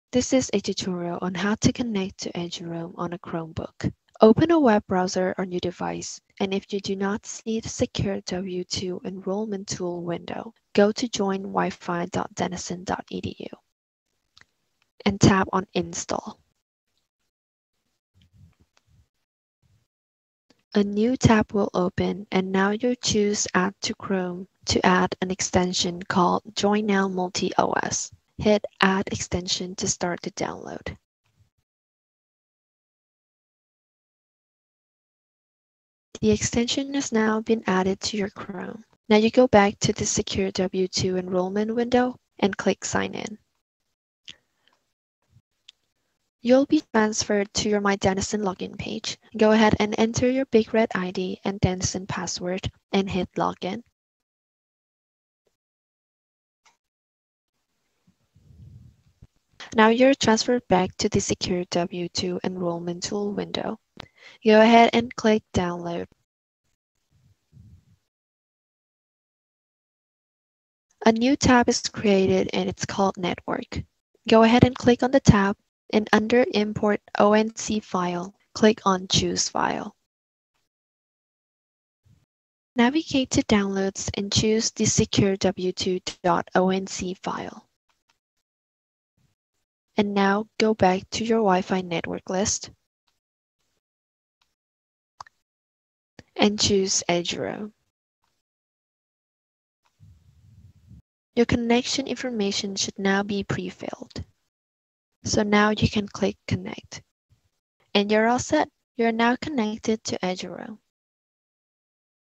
Eduroam on Chromebook
Connect to Denison's secure wireless network eduroam via user certificate on a Chromebook.